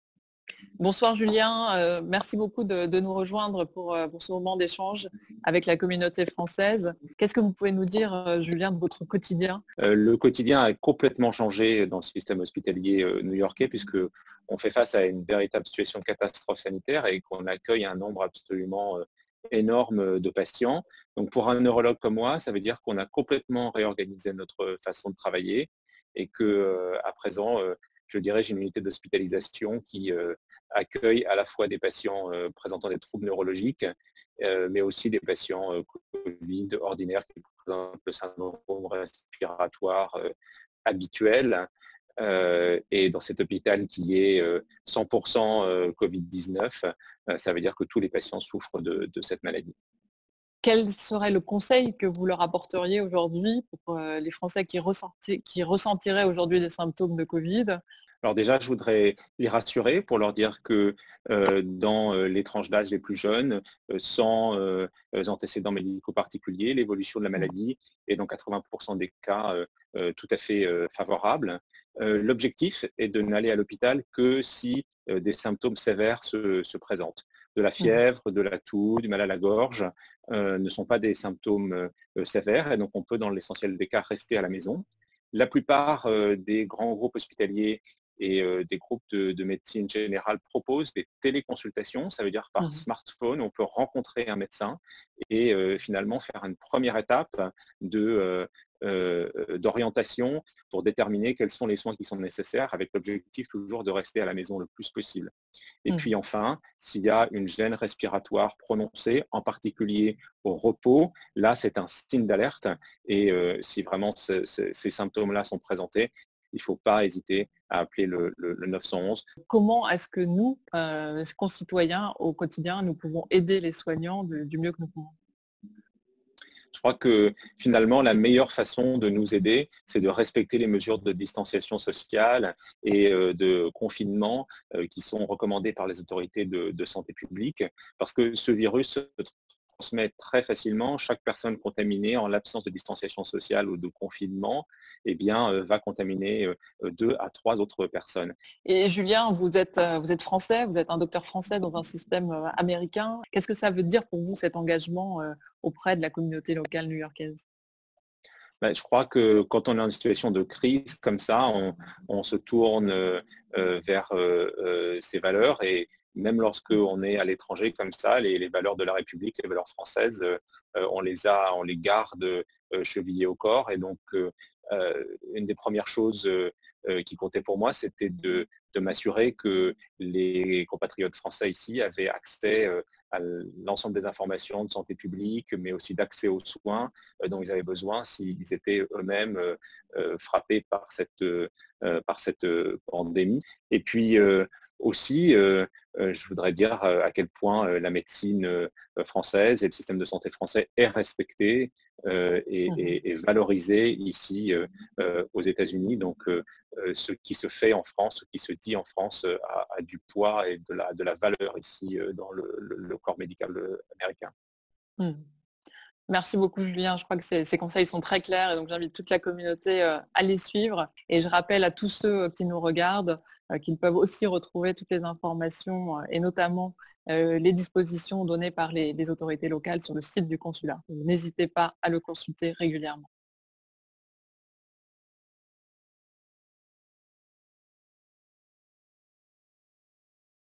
Julien Cavanagh, neurologue français au SUNY Downstate Medical Center à New York
COVID19 | "Dans une situation de crise comme celle-la, on se tourne vers ses valeurs, et même lorsqu'on se trouve à l'étranger, les valeurs de la République, les valeurs françaises, on les a, on les garde, chevillées au corps."

Julien Cavanagh est français et neurologue au SUNY Downstate Medical Center à New York, il nous raconte son quotidien dans un hôpital entièrement dédié aux patients atteints du Coronavirus et rappelle l'importance du confinement et des distances sociales.